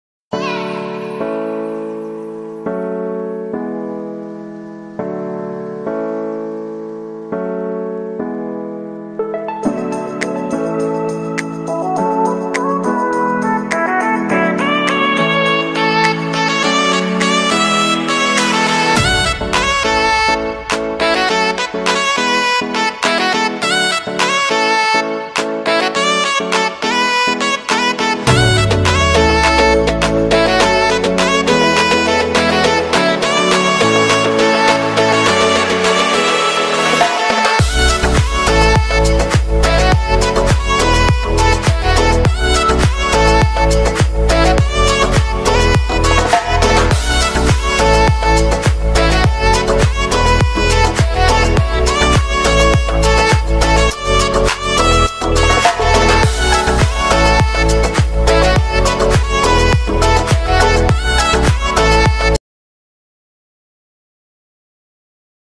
FC NAVBAHOR MEDIA TAQDIM ETADI 📽 SUPER ROLIK
“Навбаҳор”нинг Тошкентдаги ўқув-машғулот йиғинидан видеоролик. Жамоамизнинг 2 давра уйинига тайёргарлиги авжида